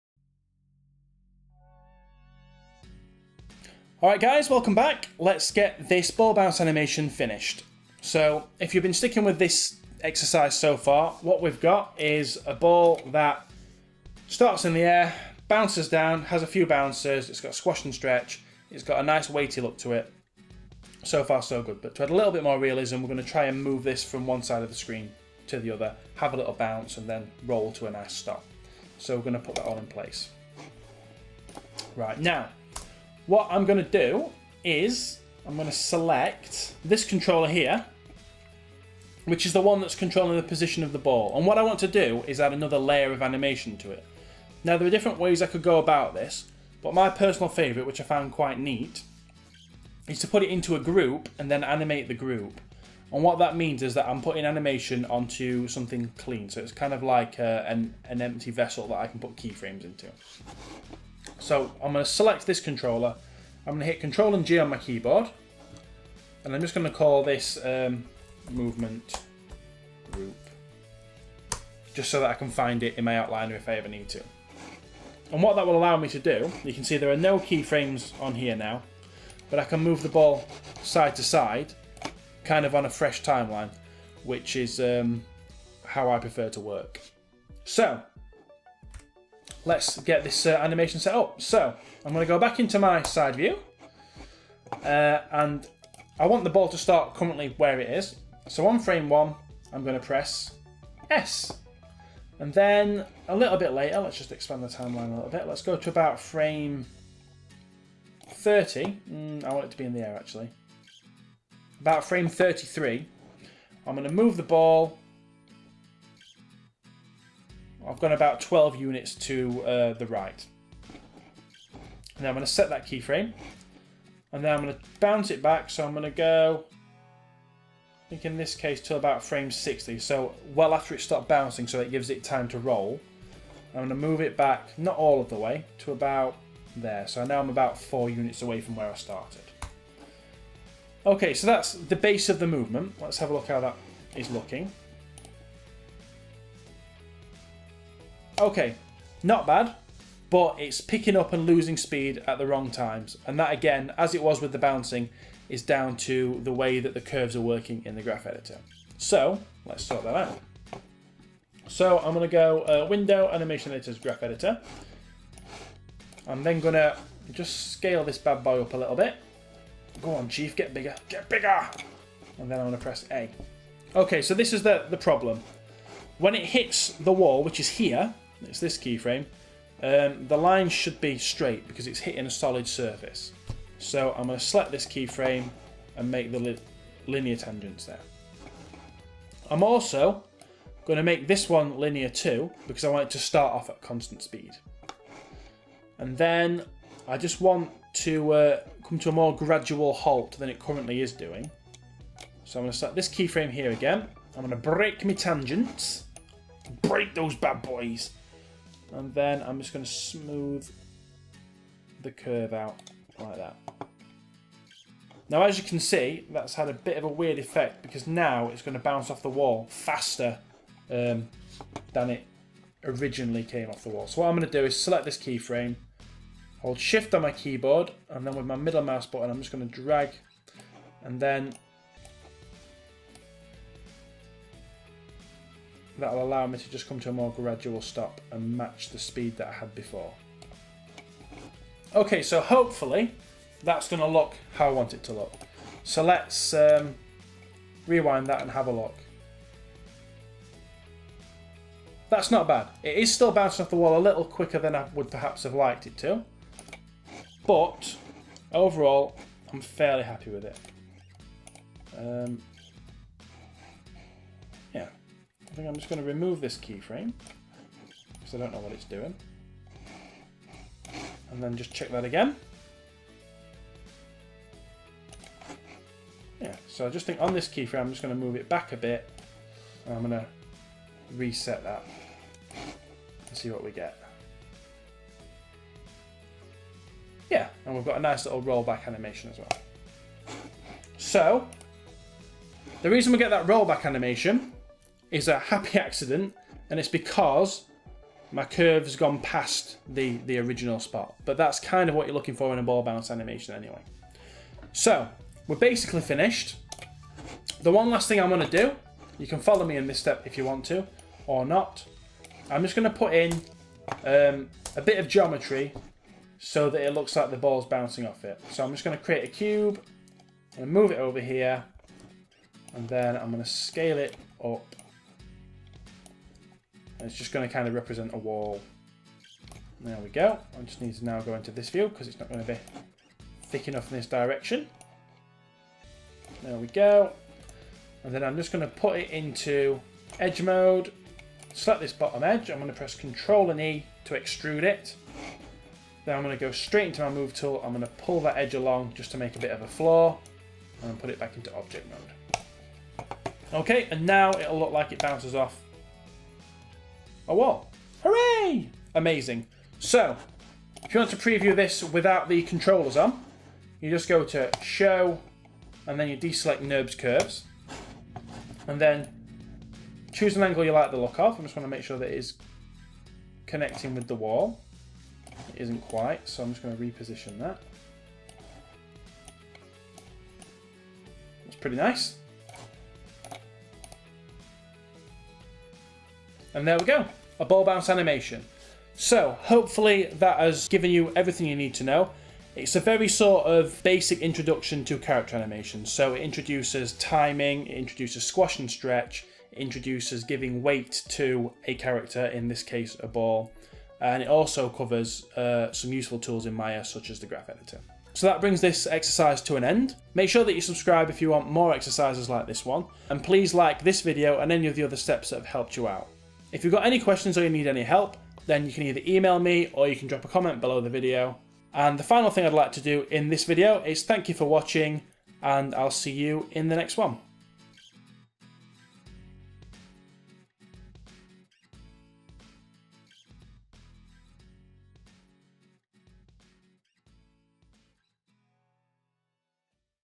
Maya Bouncing Ball Tutorial #5 Hit the Wall
In the 5th and final part of this Maya animation tutorial for beginners you will make the bouncing ball collide with a wall and then roll to a stop.